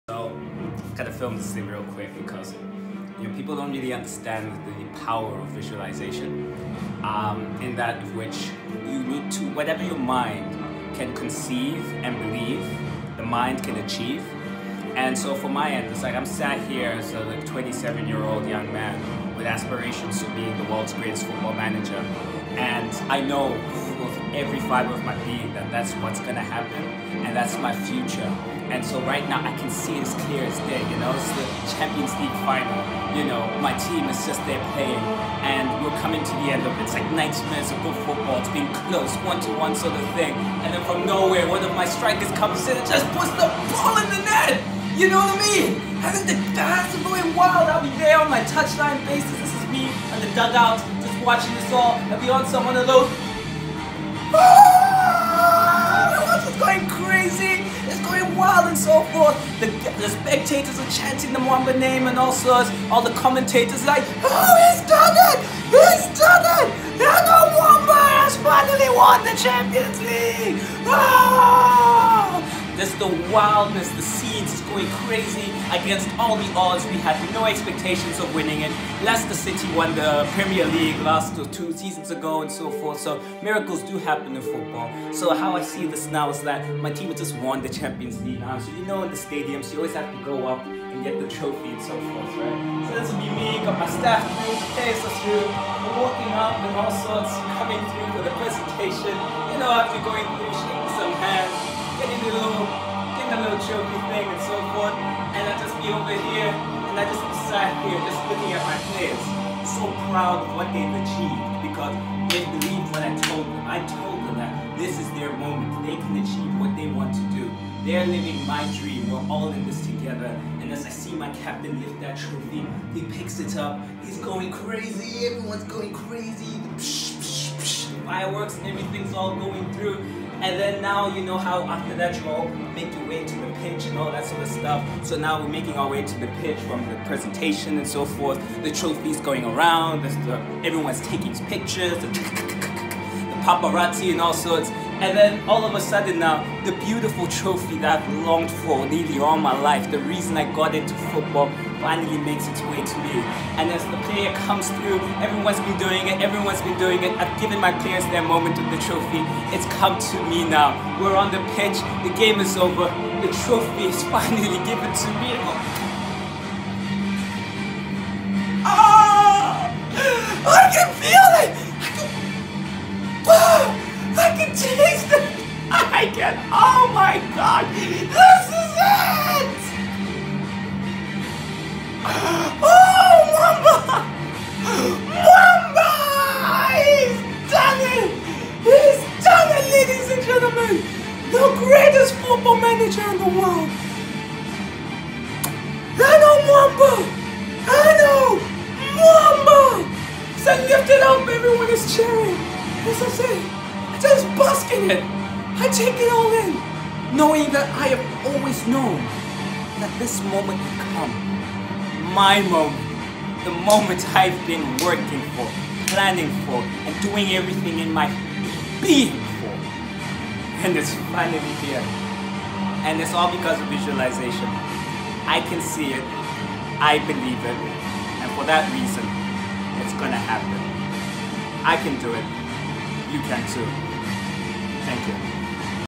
Visualise Your Success
There is no such thing as perfect timing, the challenge is for us to make the timing perfect! My name is Eno Mwamba and I dream to become the greatest football manager in the world. I currently work in finance, and stand a long way away from achieving my goals. However I love public speaking and motivating people. I think my journey to achieving my dreams is a pretty motivational one, so I seek to share it with everyone, and hopefully inspire people to go out and achieve their own dreams. Thank you so much 🙏🏾